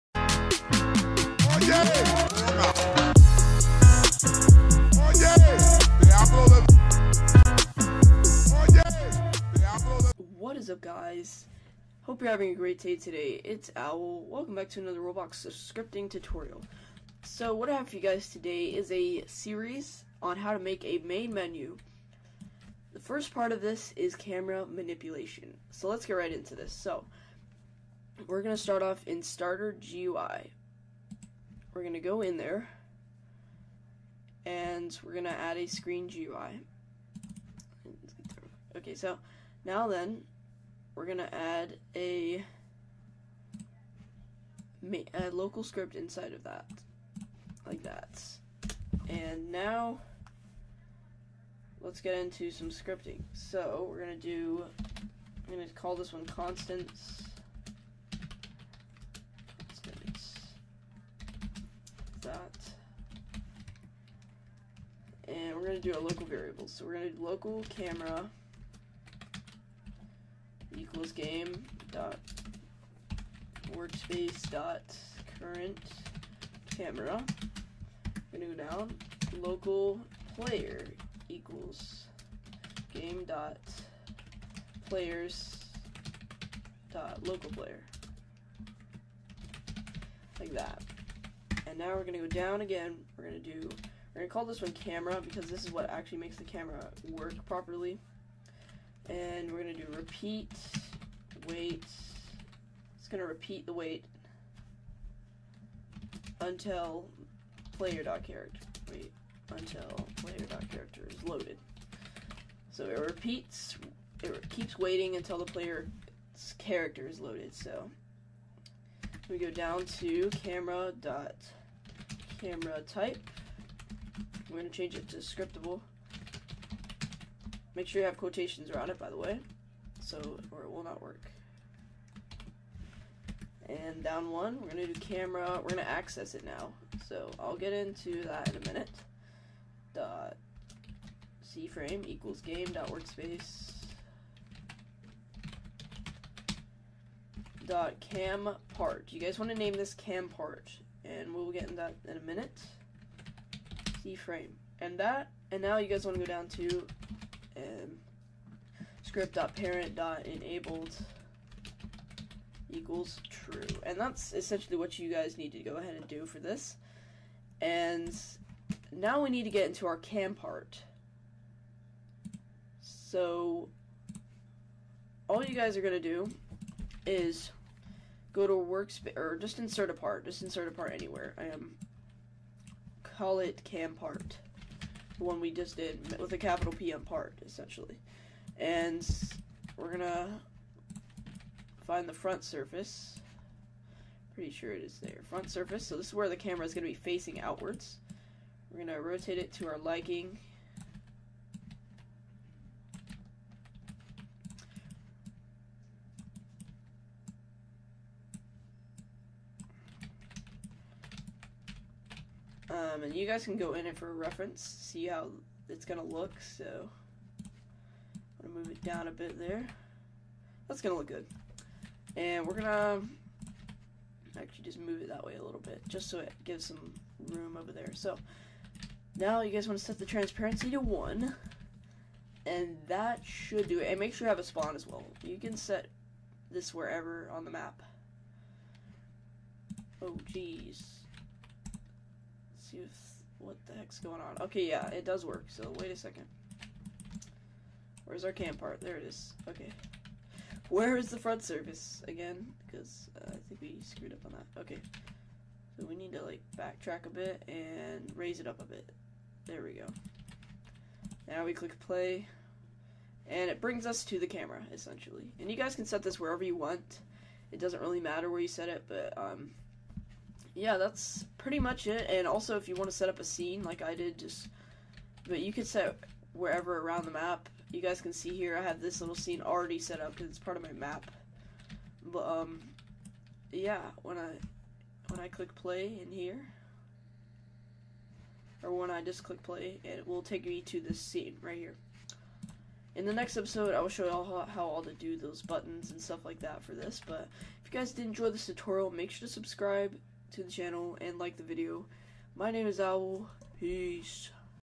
Roblox Scripting Main Menu - [PART 1] Camera Manipulation EASY!
Hey guys and welcome back to another video! In this one I showed you part 1 of my series on making a Main Menu. Part 1 is how to manipulate the camera.

Enjoyz!!